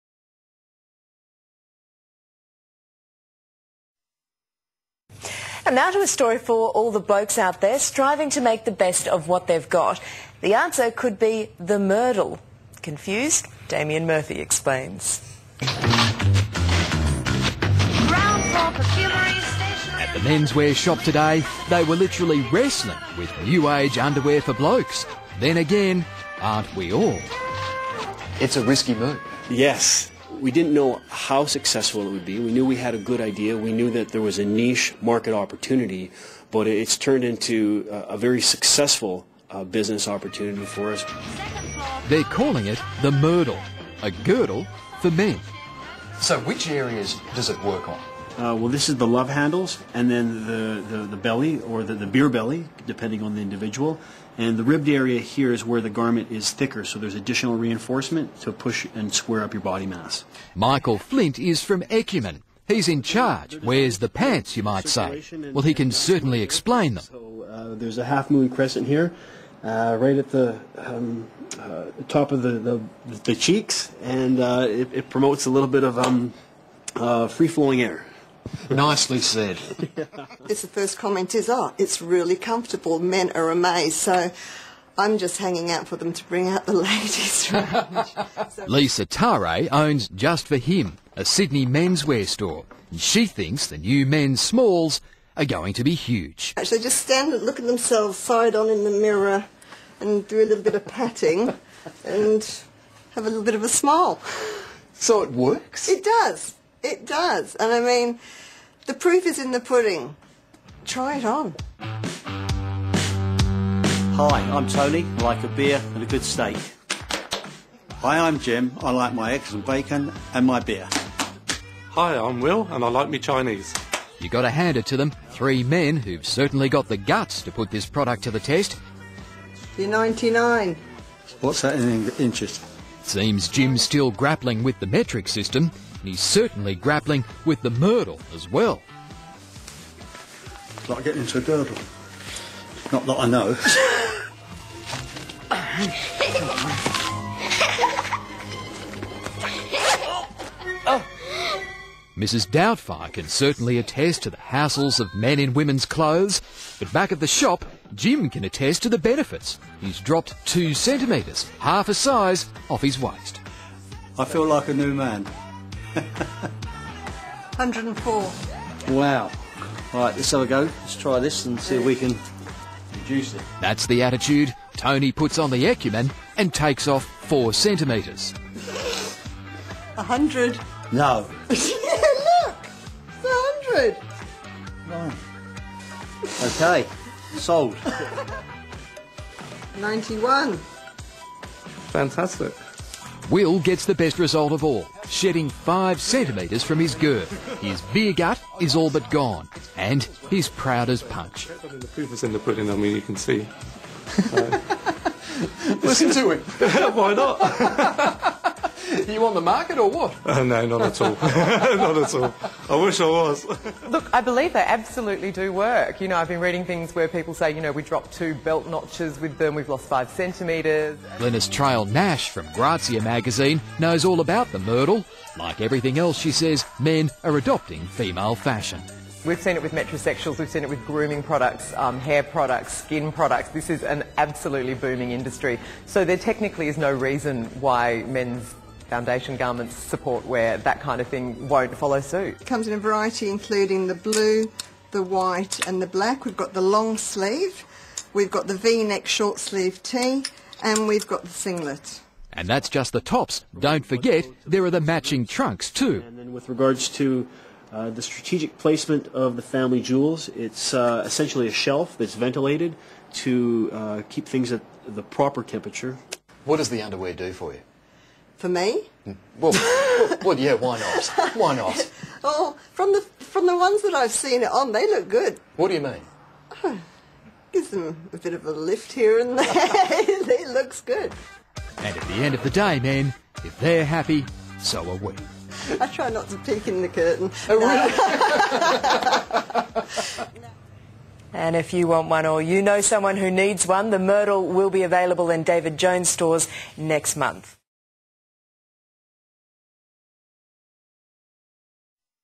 Just For Him on Ch10 News promoting Equmen
Just For Him is a shop located in Beecroft NSW.  It specialises in carrying products that are high quality and well priced.  Just For Him carries the Equmen - nicknamed 'The Mirdle" by the press. Equmen is for all men that ..
Want to look good
Have back pain
Want to control their body temperature
In fact - practically every man will benefit from wearing one
Cheers - Lisa